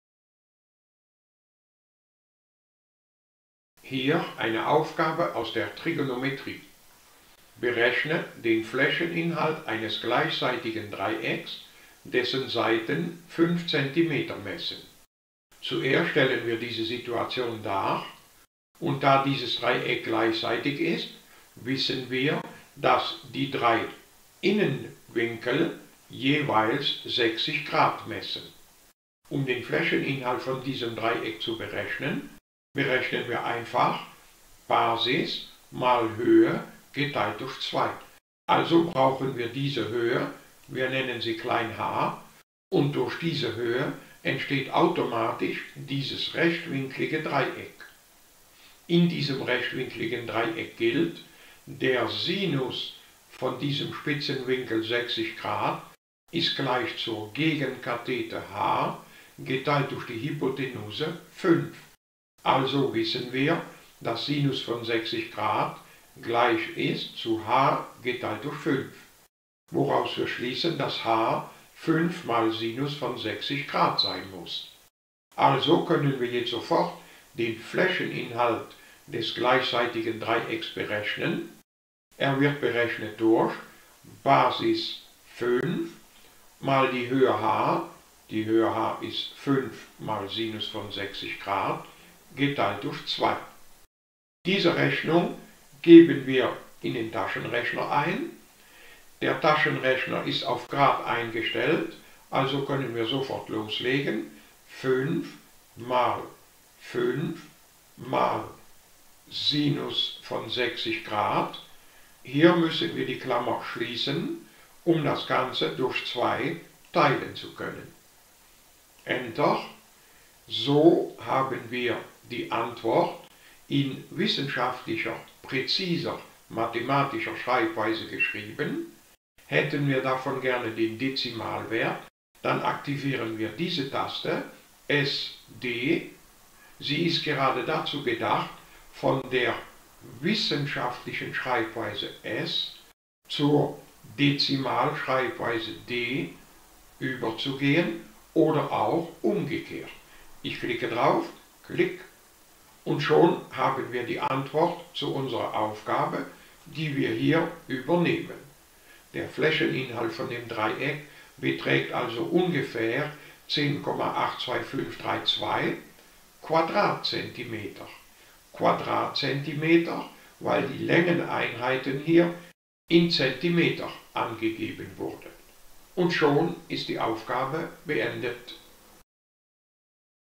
Trigo Tricks 002
Wir berechnen den Flächeninhalt eines gleichseitigen Dreiecks durch einfache Trigonometrie in einem rechtwinkligen Dreieck.